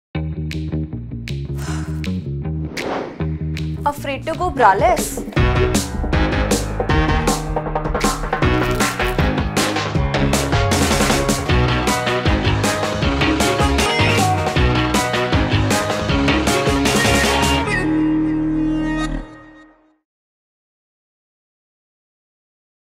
How To Use A Nipple Cover | Reusable Silicone Nipple Cover | Guide To Use Nipple Cover | Sirona
Afraid of your nipples showing through tight or sheer clothing? Well we have got reusable silicone nipple covers to give complete discretion from protruding nipples. Watch this video where our expert will show how to use these sweat-proof nipple pasties. They cover the nipple area perfectly to give a seamless look and the best part? It's skin-friendly. Now you can wear those sheer and deep neckline dresses without any hassle of wearing a bra. Get your hands on these nipple covers just from Sirona.